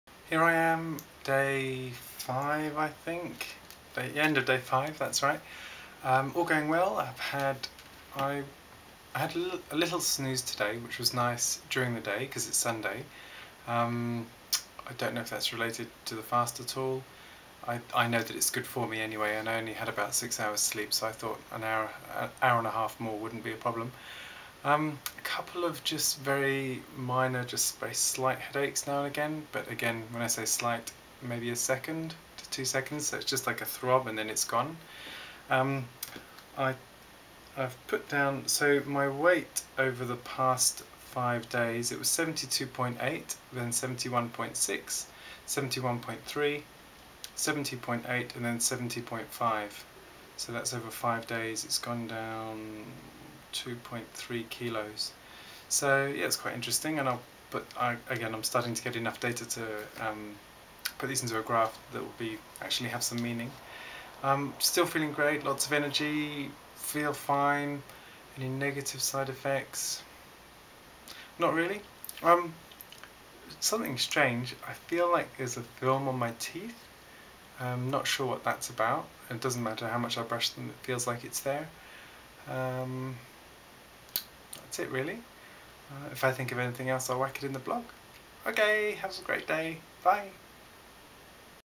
The Davinator Fasting Day 5 of 28
Today's been another enjoyable day.  Off to the organic markets this morning at 9am followed by a day pottering around the house